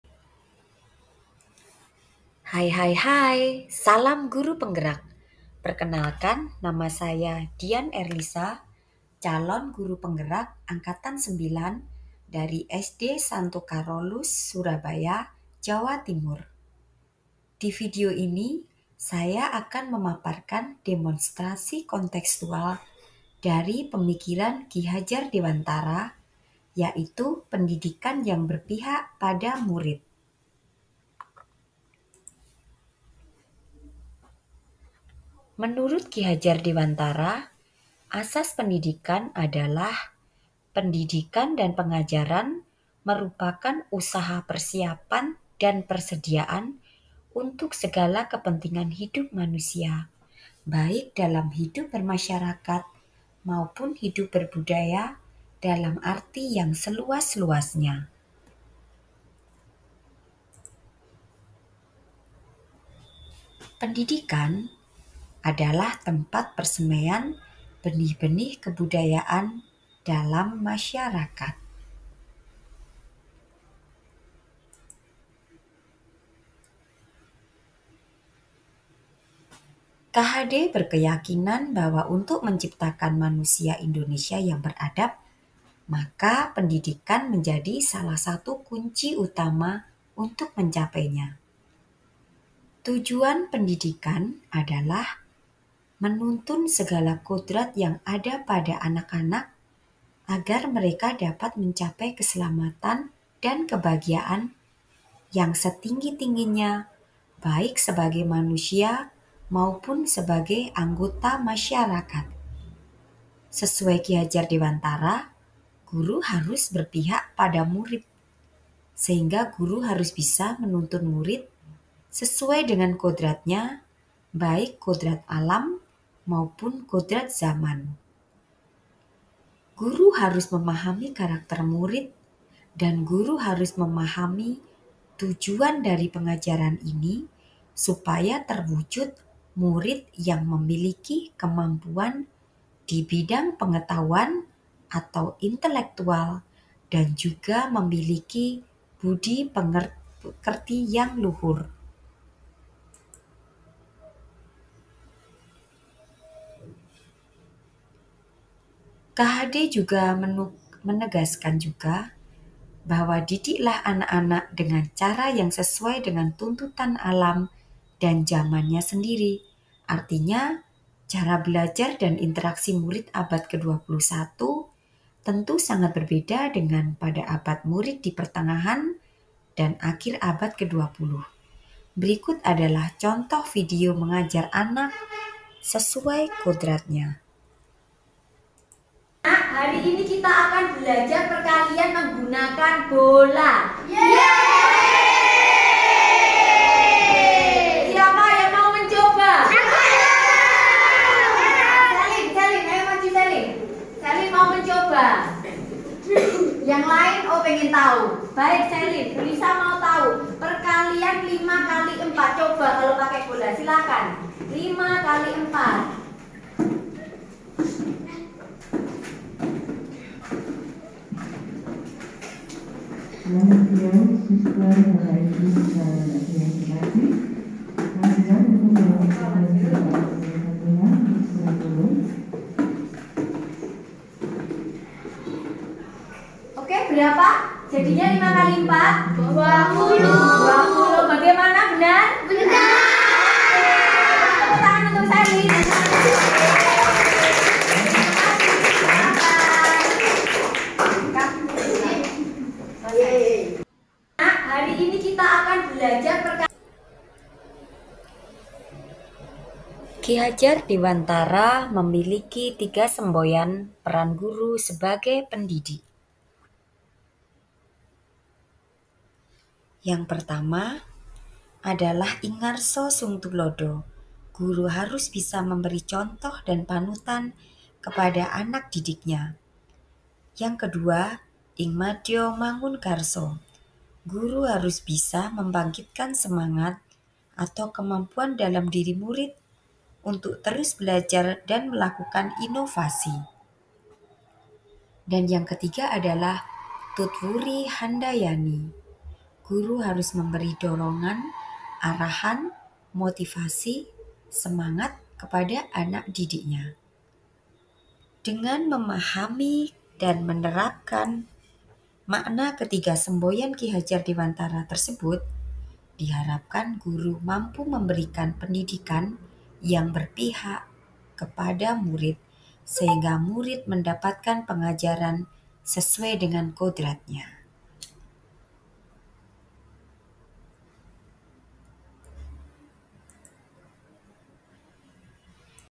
Video Demonstrasi Kontekstual Pemikiran Filosofis Ki Hadjar Dewantara
Video ini merupakan tugas demonstrasi kontekstual modul 1.1.a.5 dalam pendidikan guru penggerak berdasarkan pemikiran Ki Hajar Dewantara yaitu pendidikan yang berpihak pada murid.